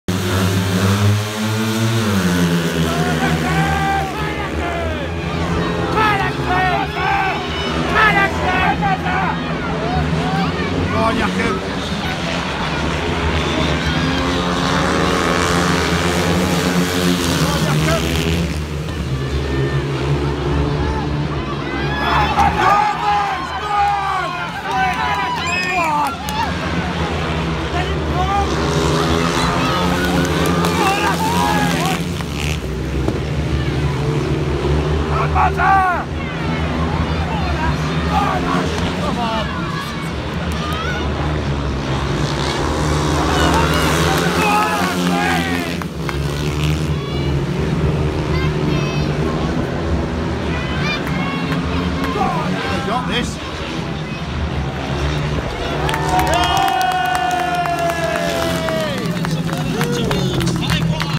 Wolves v Kings Lynn 30/04/18
Heat 1 Jacob Thorssell (56.82) Ashley Morris Niels-Kristian Iversen Michael Palm Toft 5-1 (5-1)